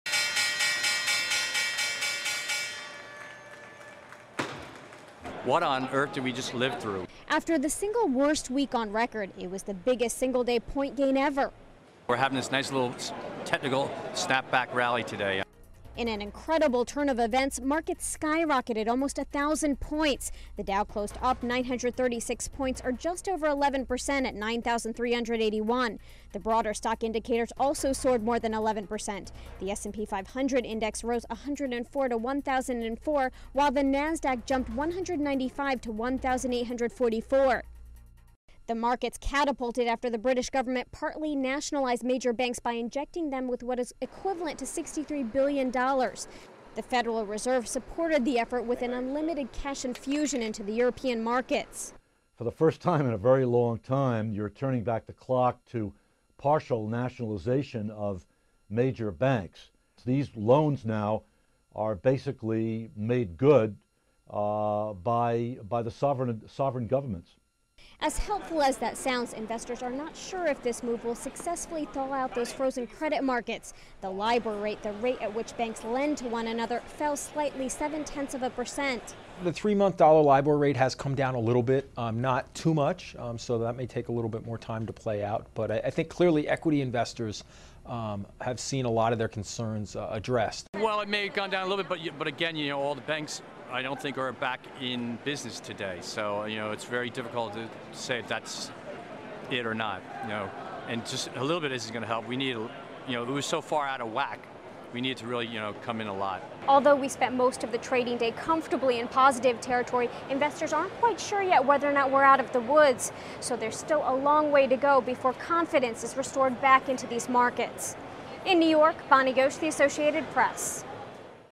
Manic Monday: Dow Soars Nearly 1,000 Points
Wall Street stormed back after its worst week ever and staged the biggest single-day stock rally since the Great Depression on Monday, catapulting the Dow Jones industrials to a 936-point gain. (Oct. 13)